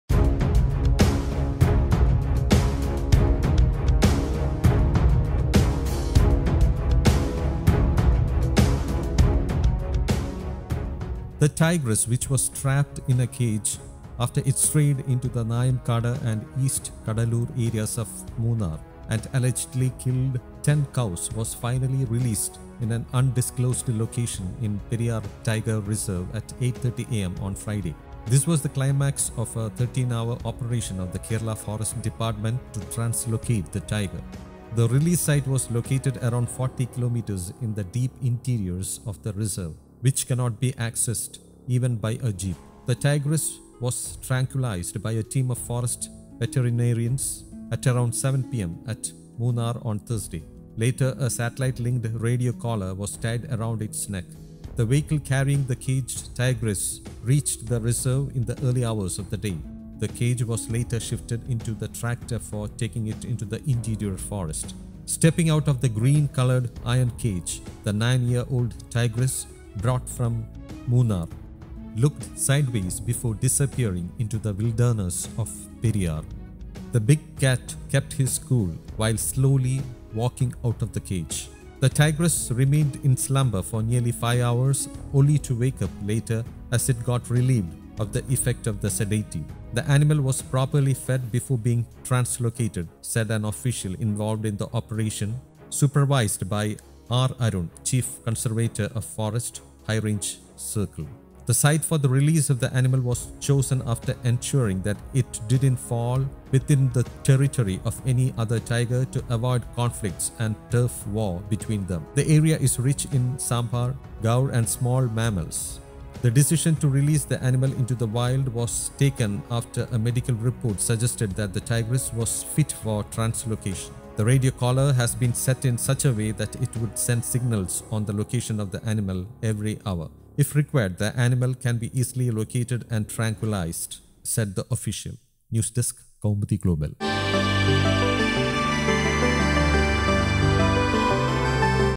Munnar tigress with satellite-linked collar released into interior forest… Watch!
The tigress which was trapped in a cage after it strayed into the Nayamkkad and East Kadalur areas of Munnar and allegedly killed 10 cows was finally released in an undisclosed location in Periyar Tiger Reserve at 8:30 am on Firday. This was the climax of a 13-hr operation of the Kerala Forest department to translocate the tiger.
The release site was located around 40 km in the deep interiors of the Reserve, which cannot be accessed even by a jeep. The tigress was tranquilised by a team of forest veterinarians at around 7 p.m. at Munnar on Thursday. Later, a satellite-linked radio-collar was tied around its neck. The vehicle carrying the caged tigress reached the Reserve in the early hours of the day. The cage was later shifted into the tractor for taking it into the interior forest.
Stepping out of the green-coloured iron cage, the nine-year-old tigress, brought from Munnar, looked sideways before disappearing into the wilderness of Periyar.